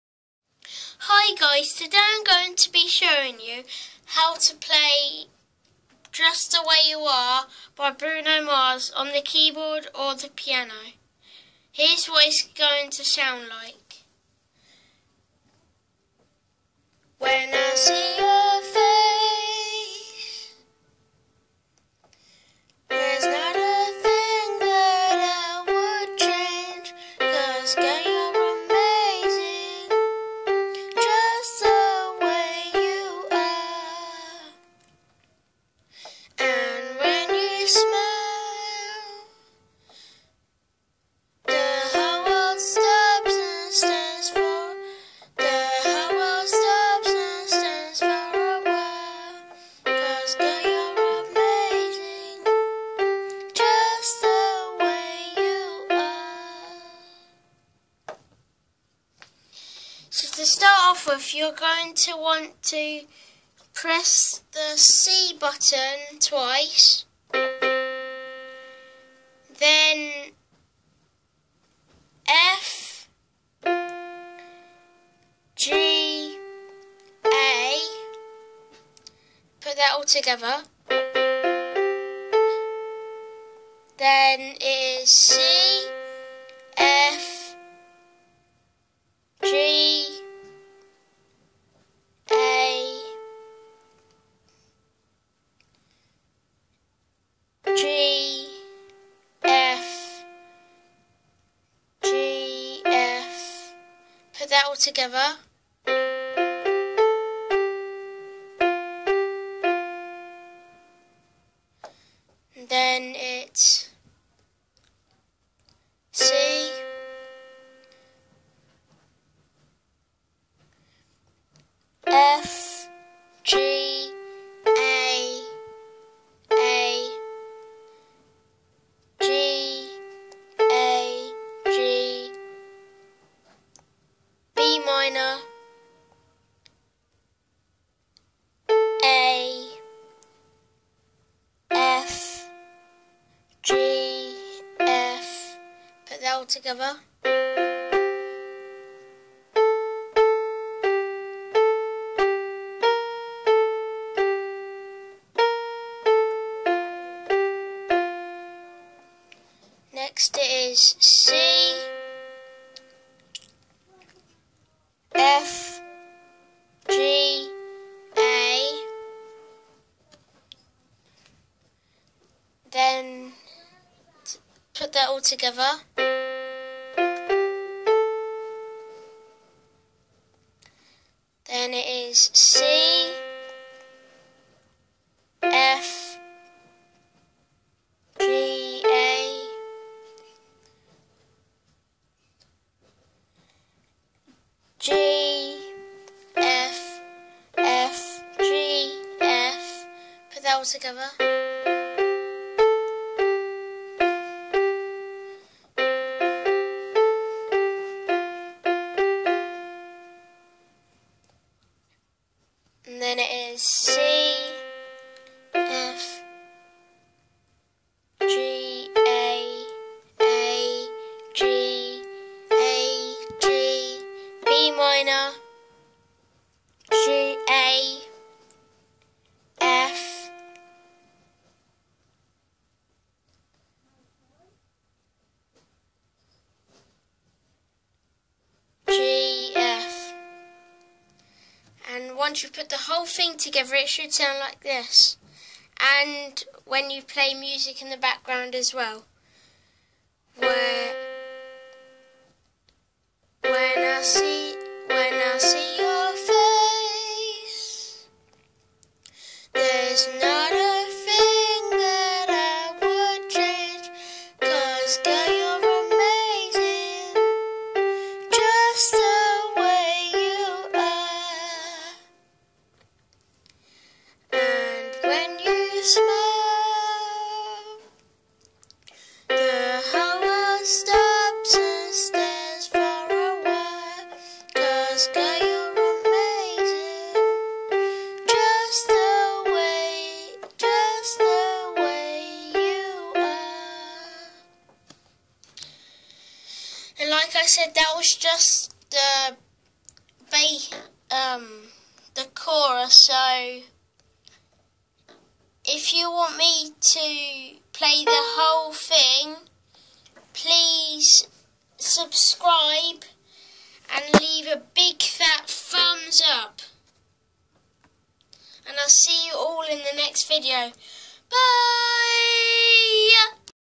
My son showing keyboard 🎹 skills and 🎶
Showing keyboard skills and singing